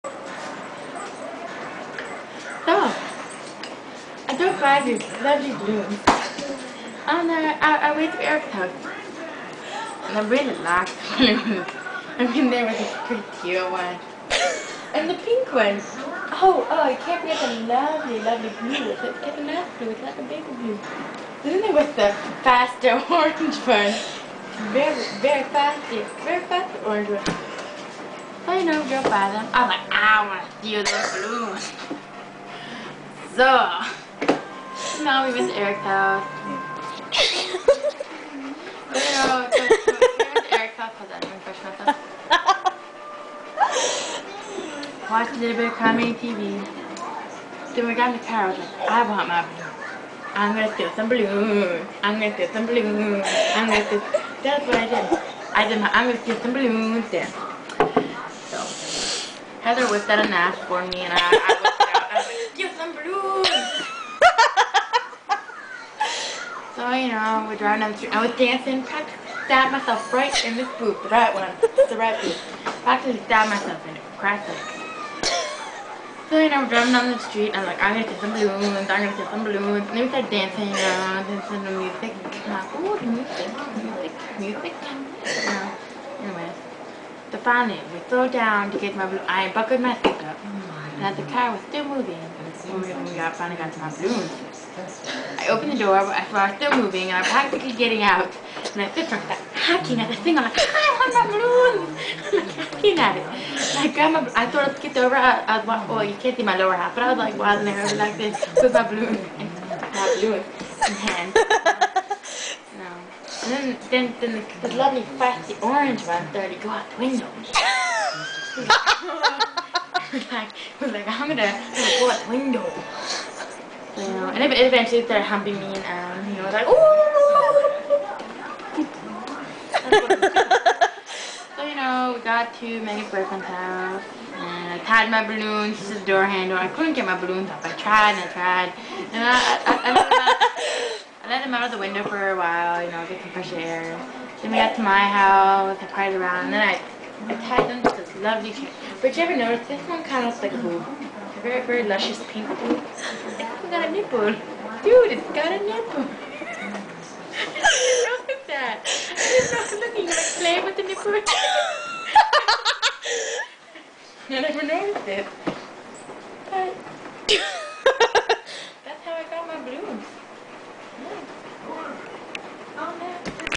Sam's balloon story.
Sam tells the story of how she gets her balloons.